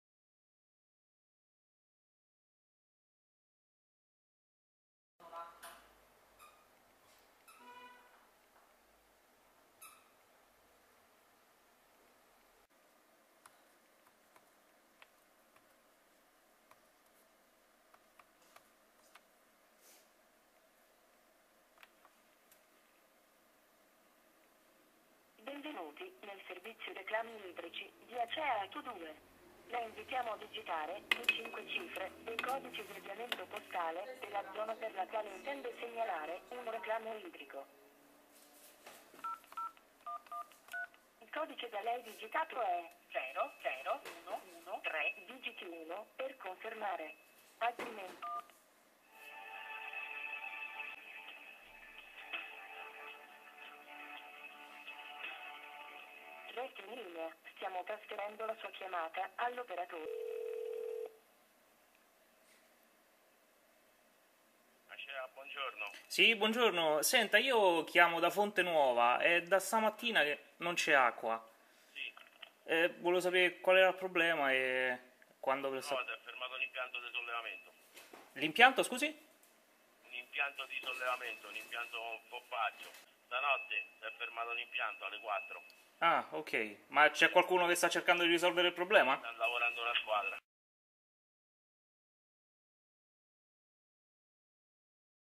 Fonte Nuova ancora senz'acqua
Continuano i problemi di natura idrica a Fonte Nuova che questa mattina si è svegliata senza un goccio di acqua dai rubinetti.
Nel video la nostra telefonata all'Acea Ato2 che lamenta un guasto alle pompe.

Diventa fan si RomaNordEst Inside su Facebook per non perdere nessun aggiornamento! Vai su e clicca su "Mi piace!"